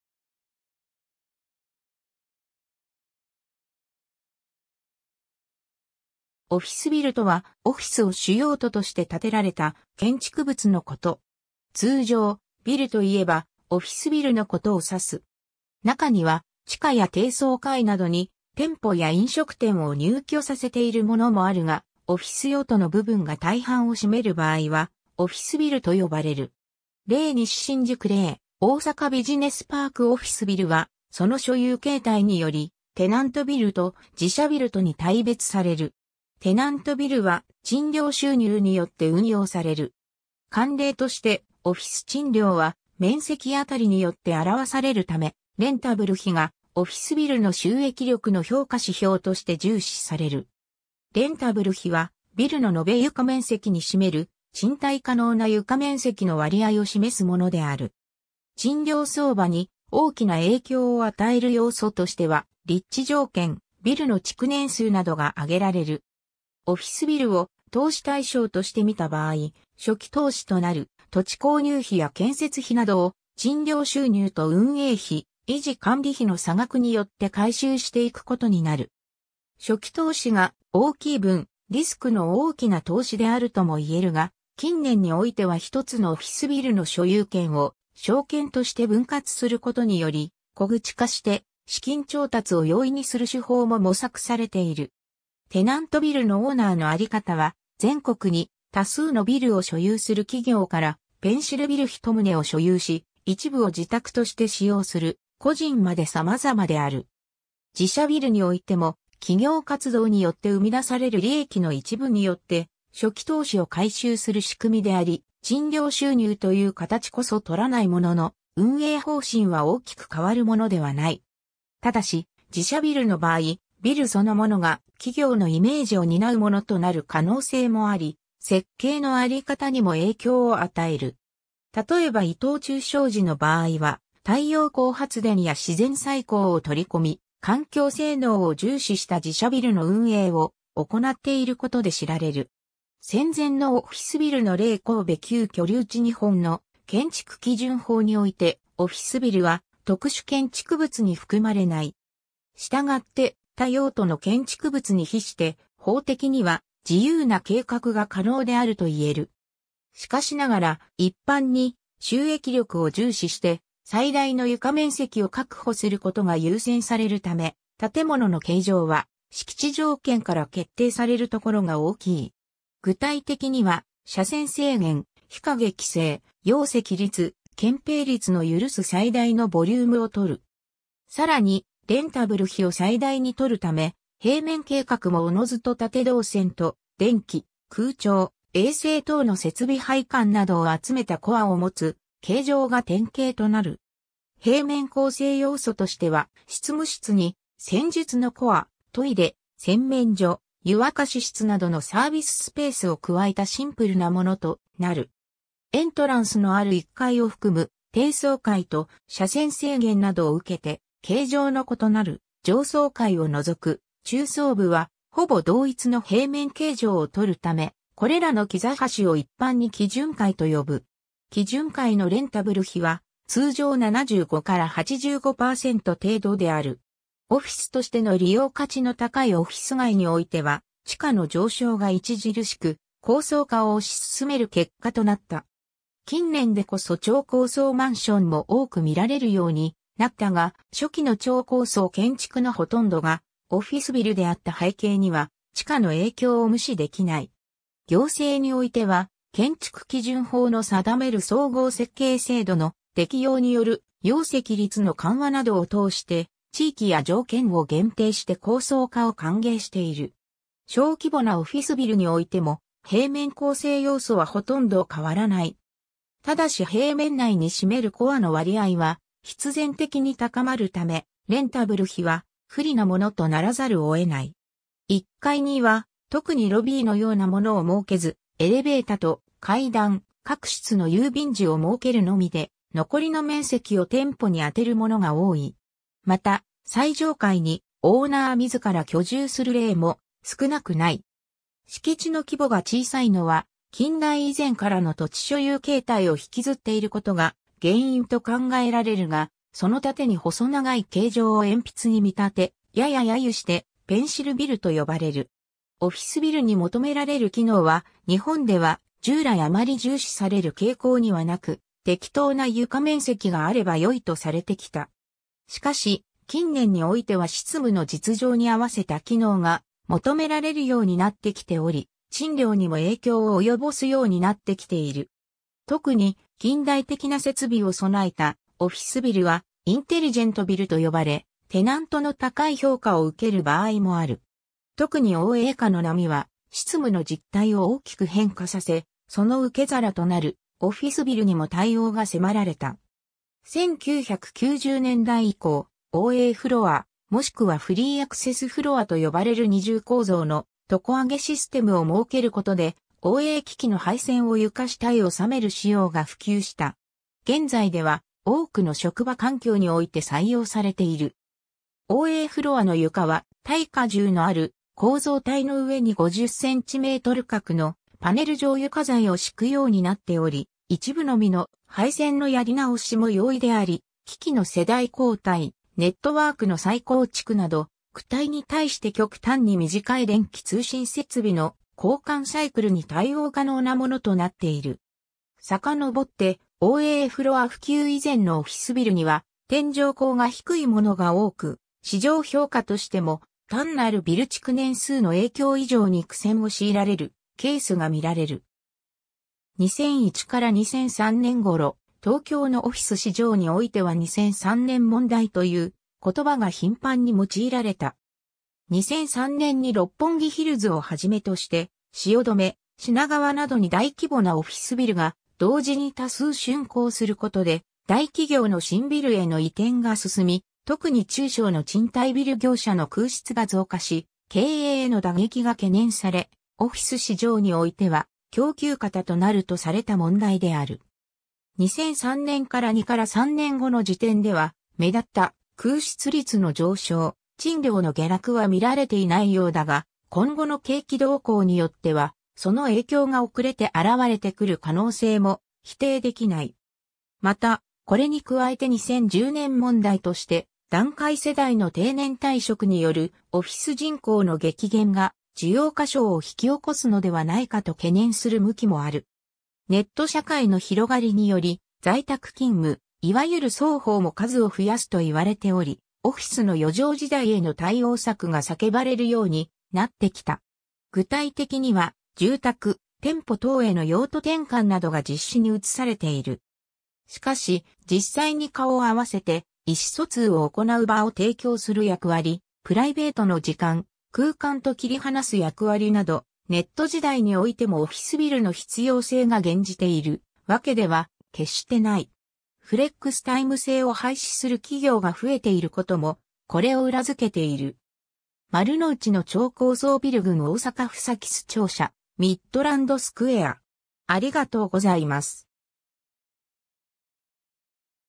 オフィスビル
オフィスビルとは、オフィス（事務所・業務）を主用途として建てられた建築物のこと。
通常、ビルといえばオフィスビルのことを指す。
 中には地下や低層階などに店舗や飲食店を入居させているものもあるが、オフィス用途の部分が大半を占める場合は「オフィスビル」（あるいは「複合オフィスビル」とも）と呼ばれる。
 例 - 西新宿（東京都新宿区） 例 - 大阪ビジネスパーク（大阪市中央区） オフィスビルは、その所有形態により、テナントビル（他者＝テナントにオフィス空間を貸し出すもの。貸しビルとも言う）と、自社ビル（所有者と使用者を同じくするもの）とに大別される。
テナントビルは、賃料収入によって運用される。
慣例としてオフィス賃料は面積あたりによって表されるため、レンタブル比（有効比、貸室面積比とも）が、オフィスビルの収益力の評価指標として重視される。
レンタブル比は、ビルの延床面積に占める賃貸可能な床面積の割合を示すものである。
賃料相場に大きな影響を与える要素としては立地条件、ビルの築年数などが挙げられる。
 オフィスビルを投資対象として見た場合、初期投資（イニシャルコスト）となる土地購入費や建設費などを、賃料収入と運営費、維持管理費（ランニングコスト）の差額によって回収していくことになる。
初期投資が大きい分、リスクの大きな投資であるとも言えるが、近年においてはひとつのオフィスビルの所有権を証券として分割することにより、小口化して資金調達を容易にする手法も模索されている。
 テナントビルのオーナーのあり方は、全国に多数のビルを所有する企業から、ペンシルビル（後述）一棟を所有し、一部を自宅として使用する個人まで様々である。
 自社ビルにおいても、企業活動によって生み出される利益の一部によって、初期投資を回収する仕組みであり、賃料収入という形こそとらないものの運営方針は大きく変わるものではない。
ただし、自社ビルの場合、ビルそのものが企業のイメージを担うものとなる可能性もあり、設計のあり方にも影響を与える。
例えば伊藤忠商事の場合は、太陽光発電や自然採光を取り込み、環境性能を重視した自社ビルの運営を行っていることで知られる。
 戦前のオフィスビルの例 - 神戸旧居留地（神戸市中央区） 日本の建築基準法において、オフィスビルは特殊建築物に含まれない。
したがって、他用途の建築物に比して、法的には自由な計画が可能であると言える。
しかしながら、一般に収益力を重視して、最大の床面積を確保することが優先されるため、建物の形状は敷地条件から決定されるところが大きい。
具体的には斜線制限、日影規制、容積率、建ぺい率の許す最大のボリュームを採る。
さらにレンタブル比を最大に取るため、平面計画も自ずと縦動線（階段室やエレベータ室、避難設備等）と電気、空調、衛生等の設備配管などを集めたコアを持つ形状が典型となる。
平面構成要素としては、執務室に先述のコア、トイレ、洗面所、湯沸室などのサービススペースを加えたシンプルなものとなる。
エントランスのある1階を含む低層階と、斜線制限などを受けて形状の異なる上層階を除く中層部は、ほぼ同一の平面形状を取るため、これらの階を一般に基準階と呼ぶ。
基準階のレンタブル比は、通常75〜85%程度である。
 オフィスとしての利用価値の高いオフィス街においては地価の上昇が著しく、高層化を推し進める結果となった。
近年でこそ超高層マンションも多く見られるようになったが、初期の超高層建築のほとんどがオフィスビルであった背景には地価の影響を無視できない。
行政においては、建築基準法の定める総合設計制度の適用による容積率の緩和などを通して、地域や条件を限定して高層化を歓迎している。
 小規模なオフィスビルにおいても、平面構成要素はほとんど変わらない。
ただし平面内に占めるコアの割合は必然的に高まるため、レンタブル比は不利なものとならざるを得ない。
1階には特にロビーのようなものを設けず、エレベータと階段、各室の郵便受を設けるのみで、残りの面積を店舗に当てるものが多い。
また、最上階にオーナー自ら居住する例も少なくない。
敷地の規模が小さいのは、近代以前からの土地所有形態を引きずっていることが原因と考えられるが、その縦に細長い形状を鉛筆に見立て、やや揶揄してペンシルビルと呼ばれる。
 オフィスビルに求められる機能は、日本では従来あまり重視される傾向にはなく、適当な床面積（一人当たり5〜8平方メートル程度といわれている）があれば良いとされてきた...